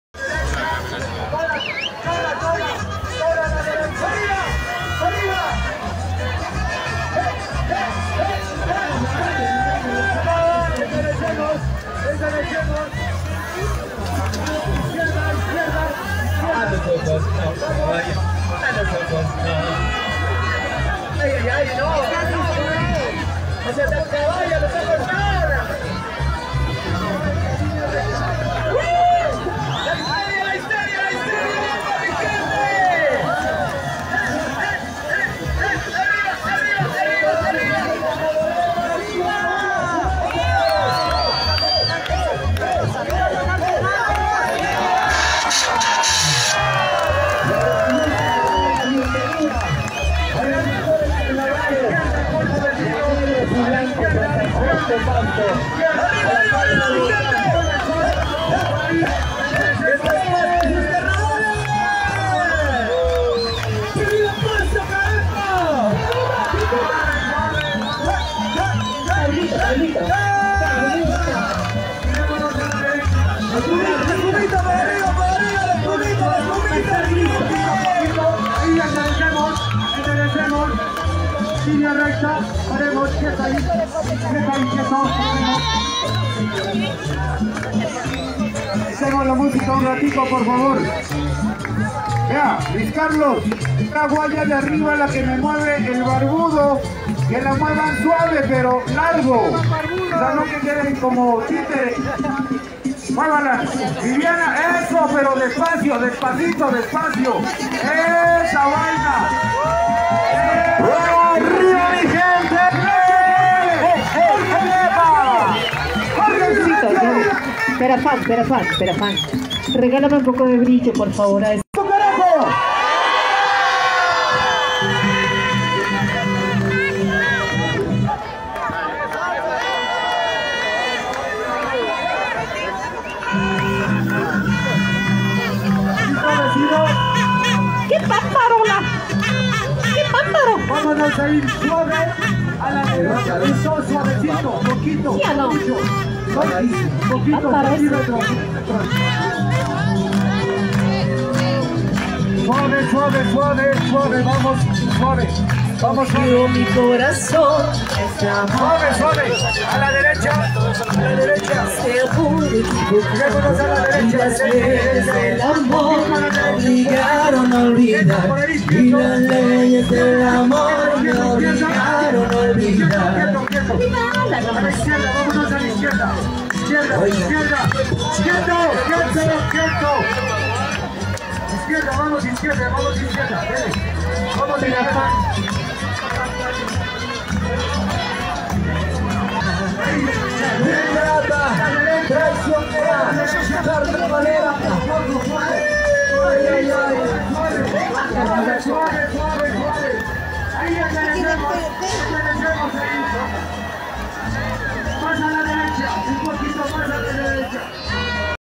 Carroza 'Ohne Titel' ocupó el séptimo puesto en el carnaval de Pasto
La carroza 'Ohne Titel', del maestro Jairo Andrés Barrera, ocupó el séptimo puesto en el carnaval de negros y blancos de Pasto. Video: José Basante Muñoz